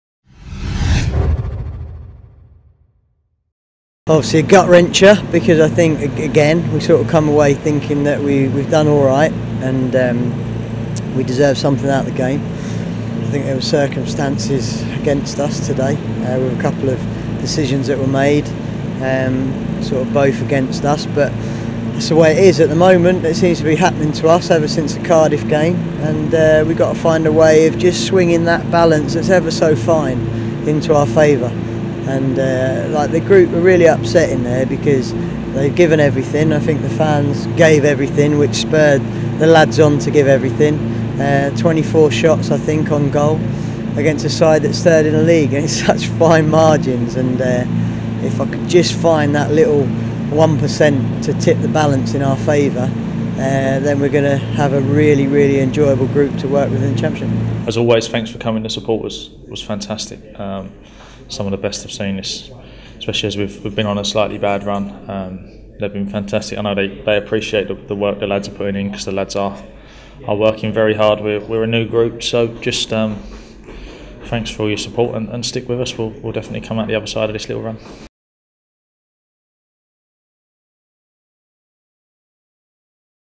Reading 2-1 Bristol City Post-Match Interviews
Lee Johnson and Gary O'Neil reflect on a disappointing afternoon at Reading.